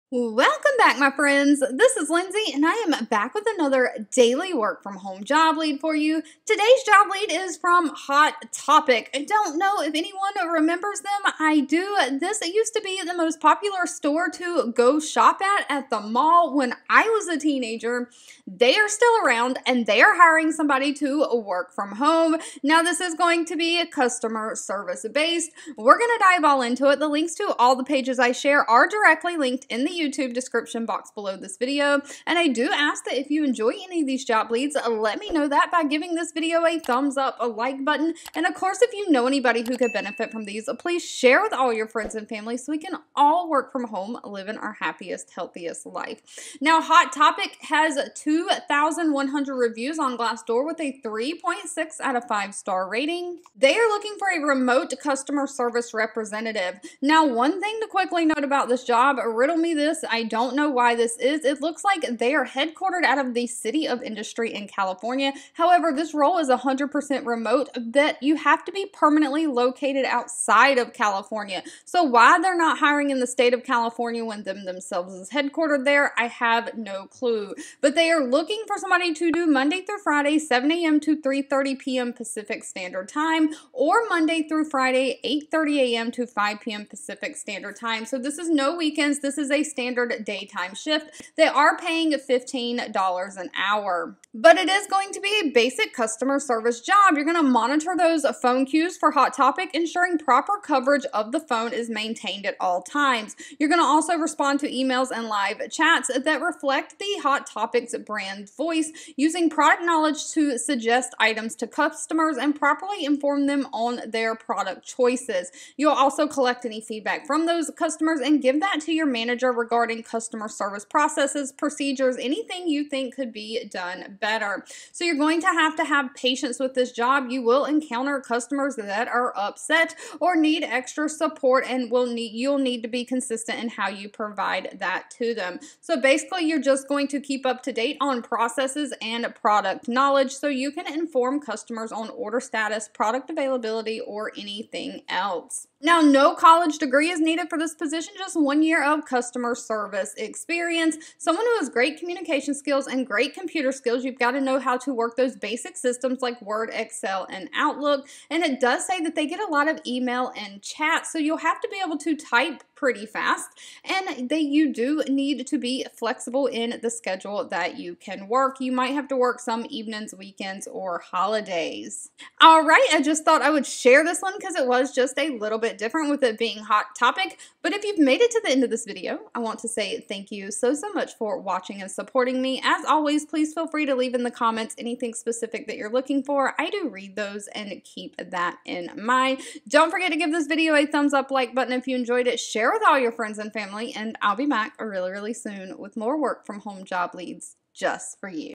HOT TOPIC Hiring Remote Work From Home Job | $120 Day | No College Degree Needed | USA Only
If you remember the malls back in the day Hot Topic was the store for people to shop at. They are hiring someone to work remotely from home doing customer service work and they don’t require any college degree. They are hiring within the United States outside of California and are paying $120 a day. Links to apply are below.